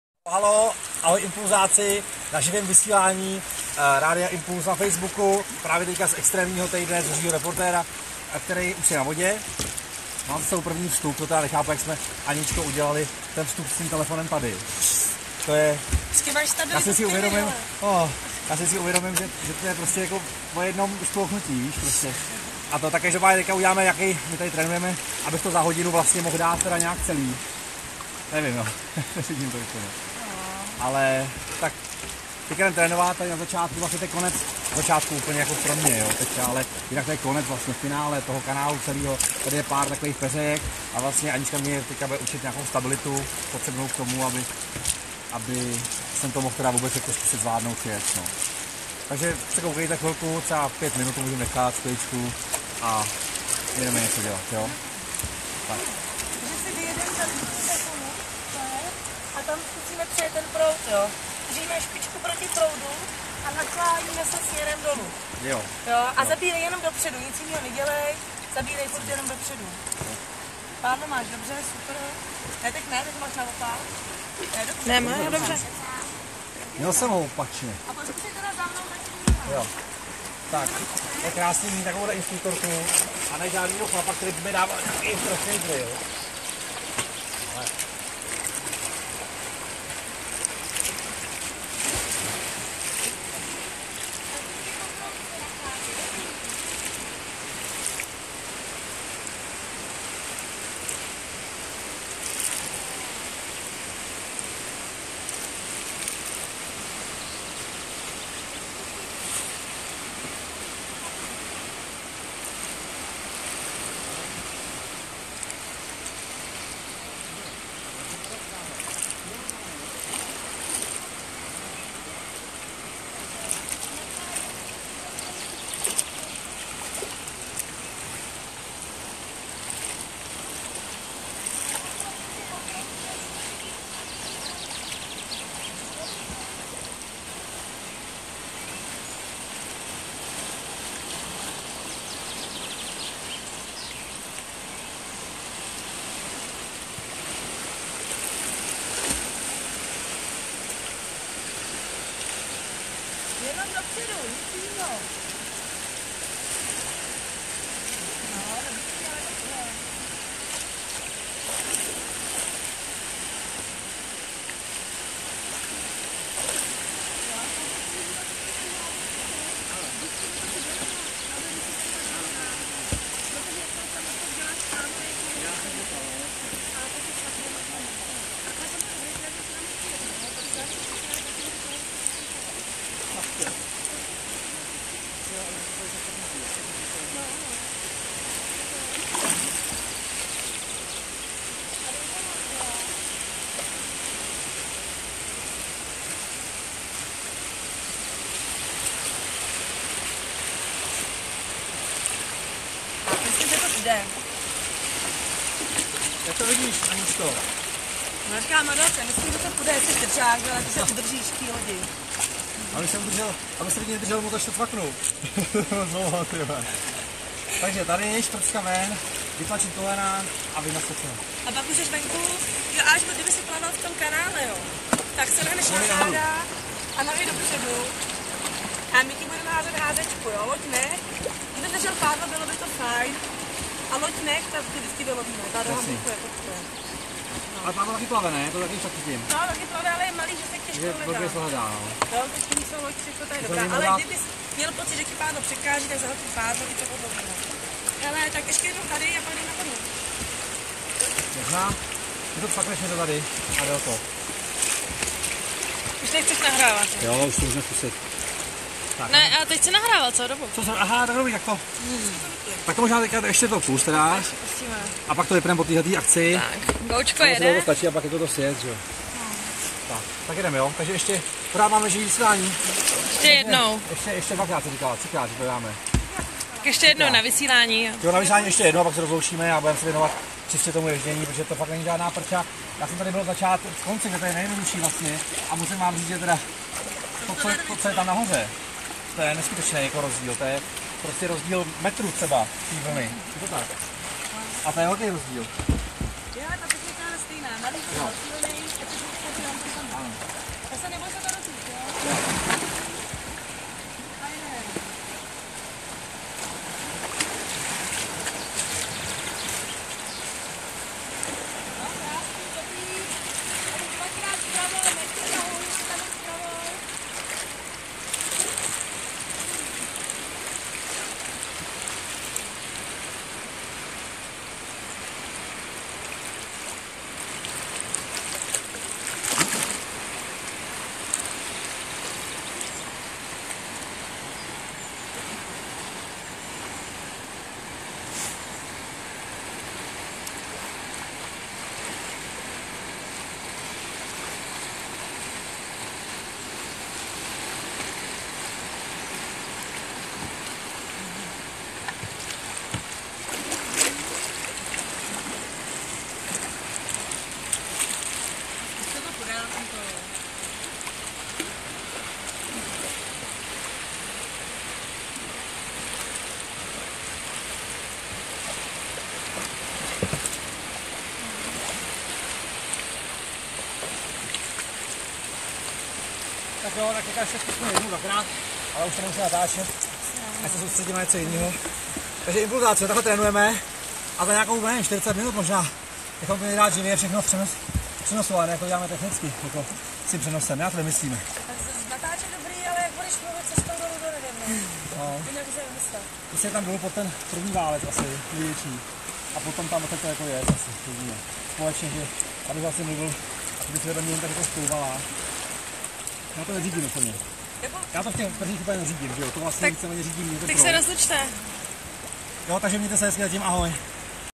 Zuřivý reportér zdolává divokou vodu
Zuřivý reportér pokračuje ve svém adrenalinovém týdnu, a to sjezdem divoké vody v kajaku. Jak se mu dařil tento sport se můžete podívat ve videozáznamu.